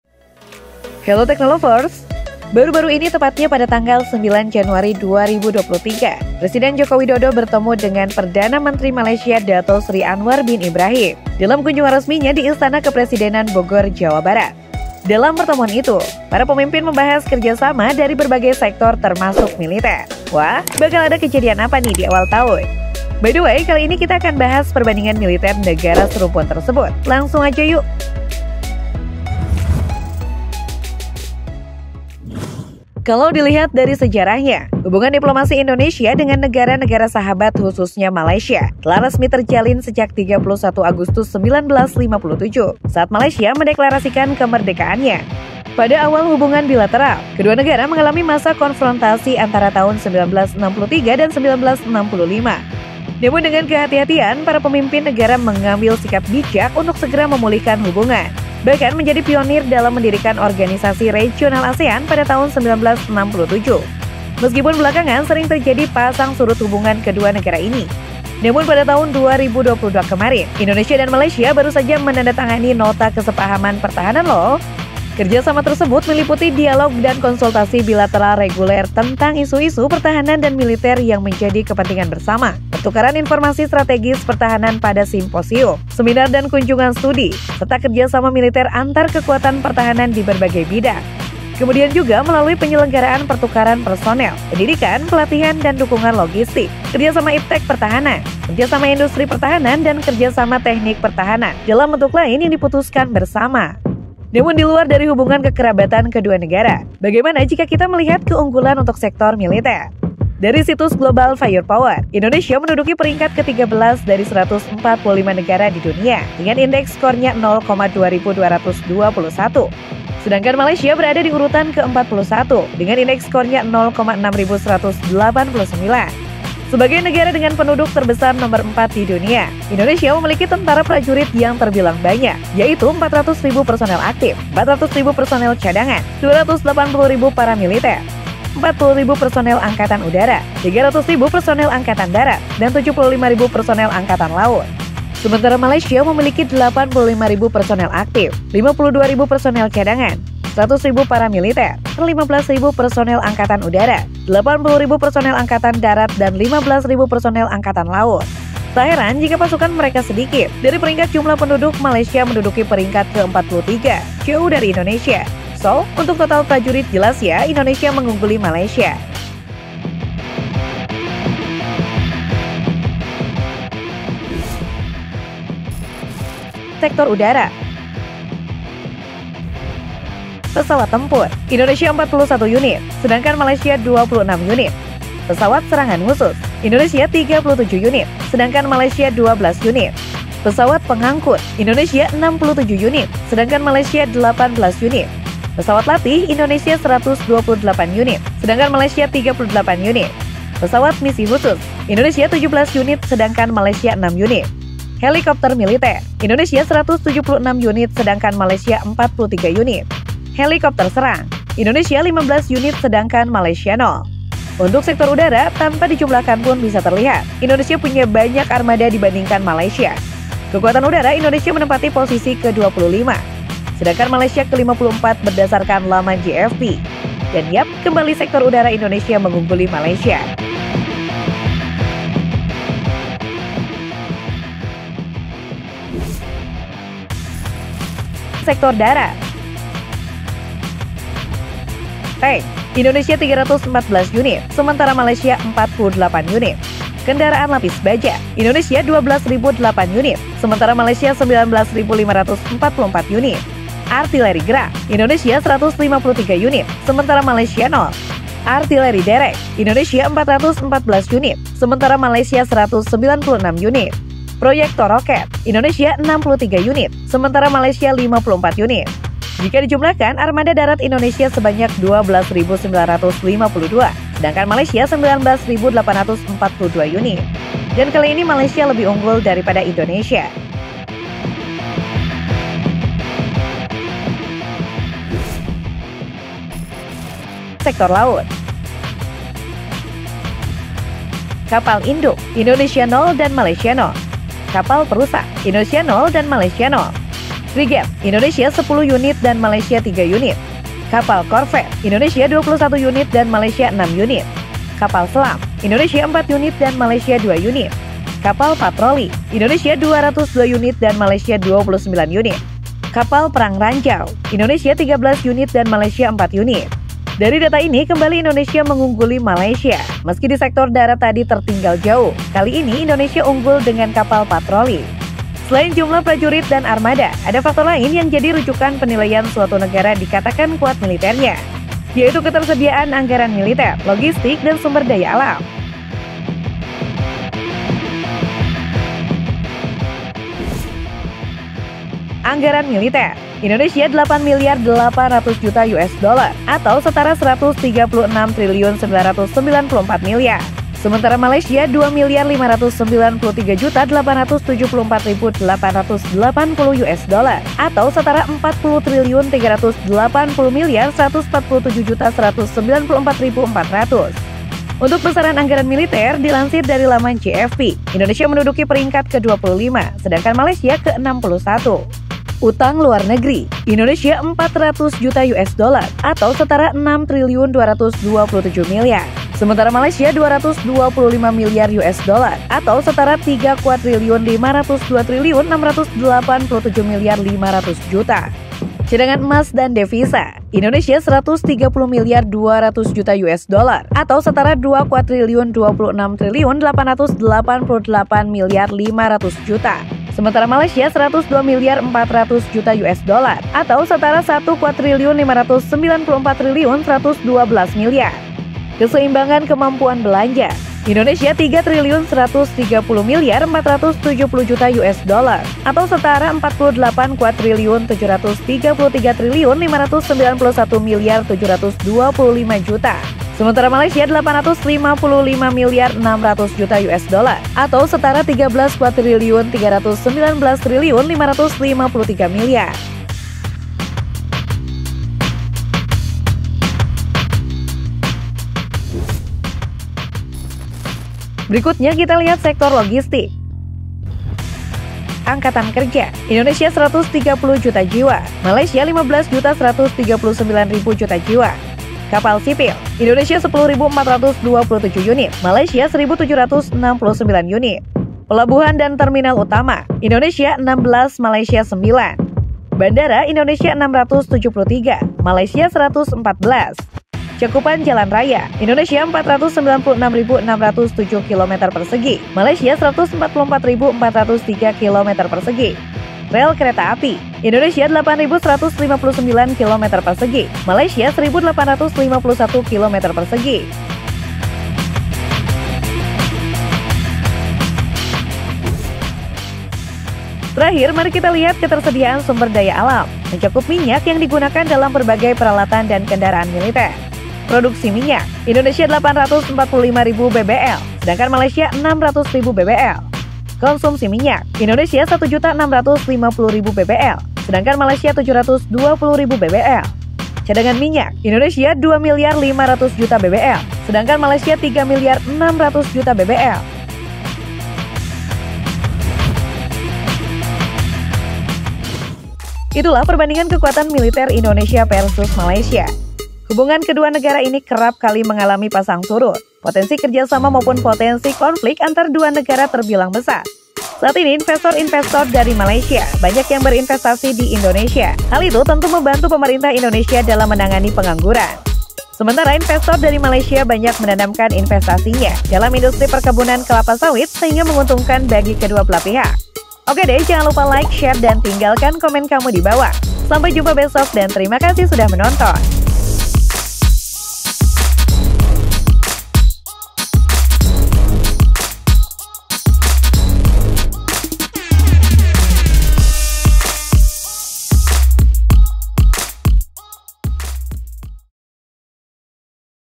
MENDADAK JADI PENGUASA ASIA TENGGARA! Lihat Perbandingan Kekuatan Militer Indonesia Vs Malaysia 2023
HELLO TEKNOLOVERS!!
Apa nih yang ada dibenak kamu, kalau mendengar “Negara Serumpun”? Iyes, Indonesia dan Malaysia. Kedua negara ini, sama-sama terletak di Asia Tenggara, dan kerap melakukan hubungan kerjasama, bahkan saling bantu jika menghadapi kesulitan. Namun ada hal menarik yang bisa kita ulik, dari keduanya. Yaitu soal militer. Bagaimana sih kekuatannya jika kita adu dari berbagai lini? Yuk simak ulasan aku, Perbandingan Kekuatan Militer Indonesia vs Malaysia.